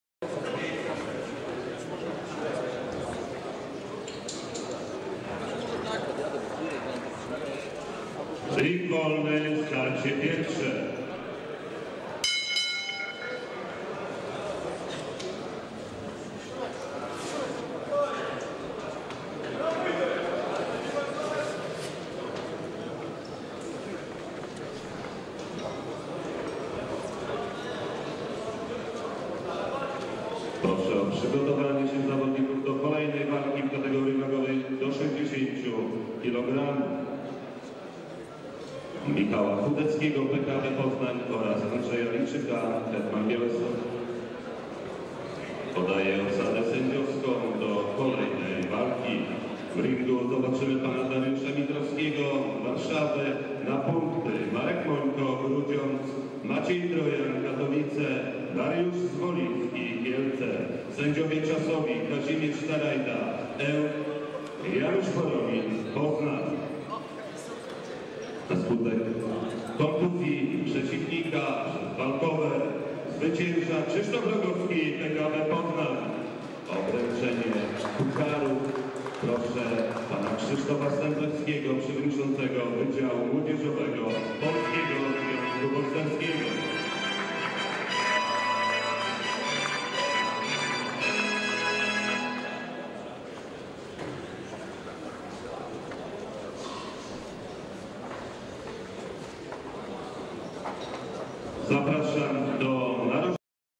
Finał 56kg walka 3 Rogowski vs Jędrzejewski Grand Prix 2010 Elbląg
Zapraszamy do obejrzenia relacji z Grand Prix PZB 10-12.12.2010.
Waga 56kg  Krzysztof Rogowski (PKB Poznań) - Marek Jędrzejewski (PKB Poznań).
Hala MOS przy ul. Kościuszki 77a  w Elblągu. W imprezie wzięło udział 54 zawodników z 21 klubów.